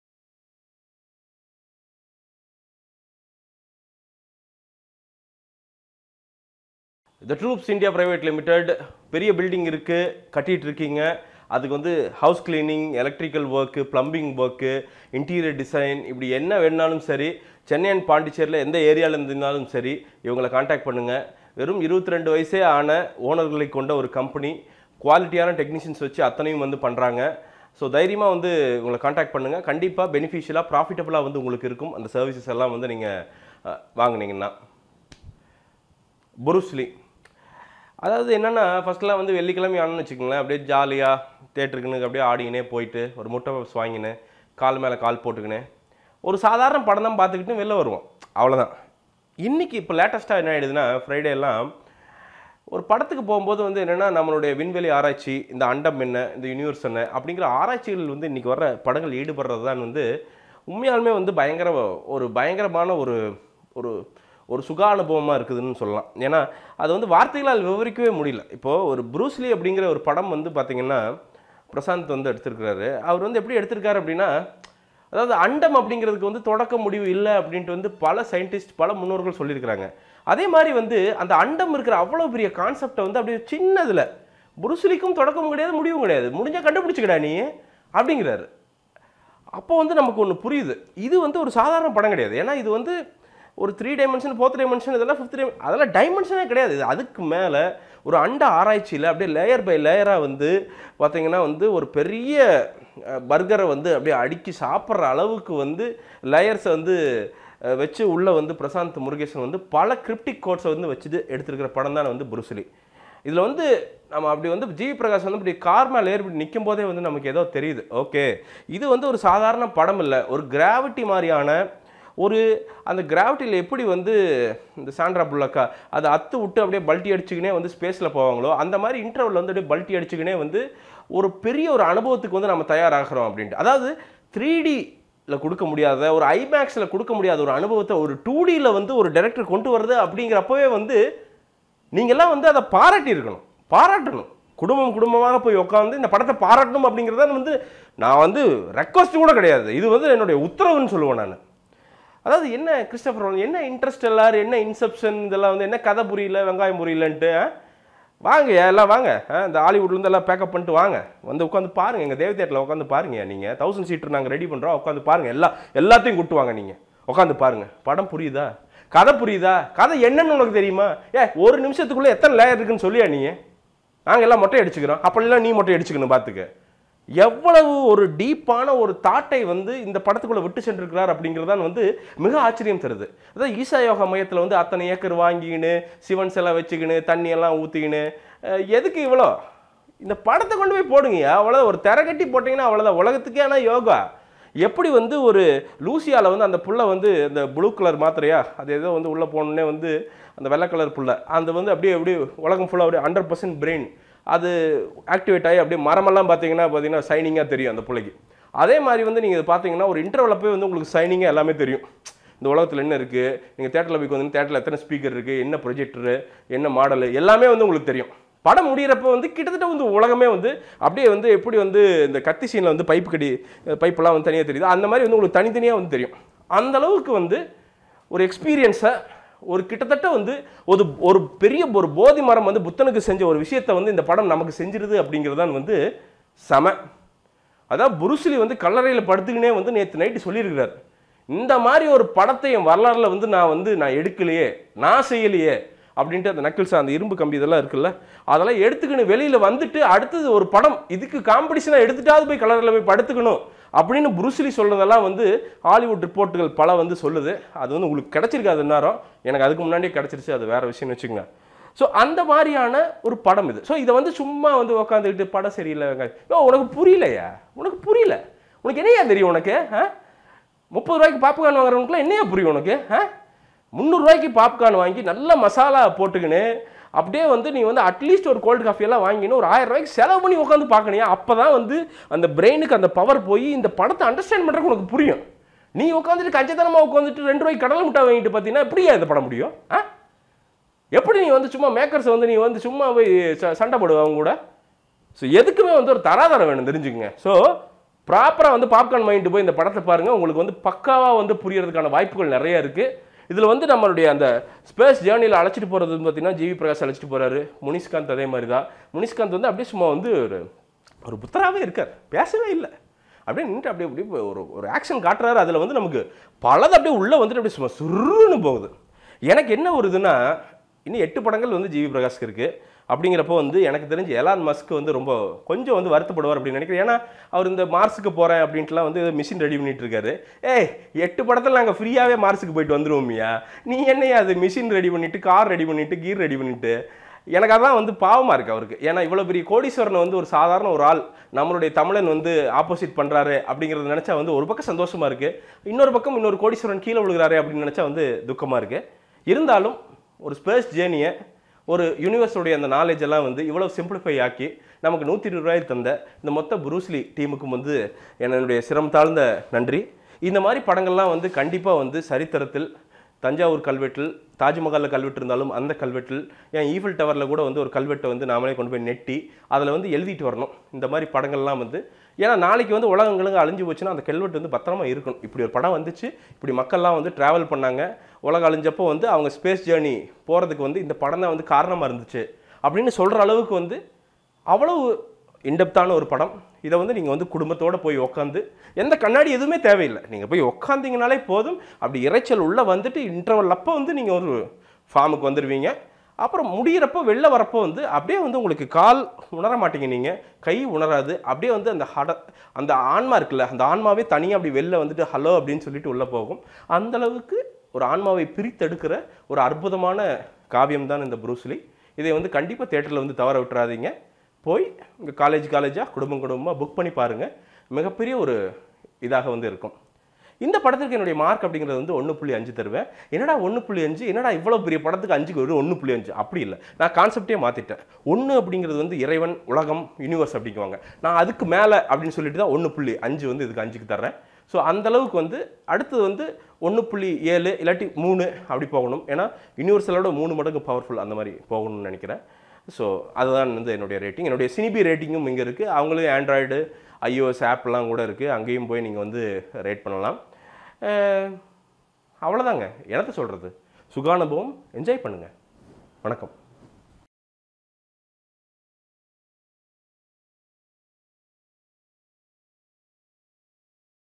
Bruce Lee - A rettha boomi review by prashanth
Review about recently released tamil movie bruce lee which has GV Prakash, Munees kanth ramadoss, bala saravanan and many other playing important roles. The movie is directed by Prashanth.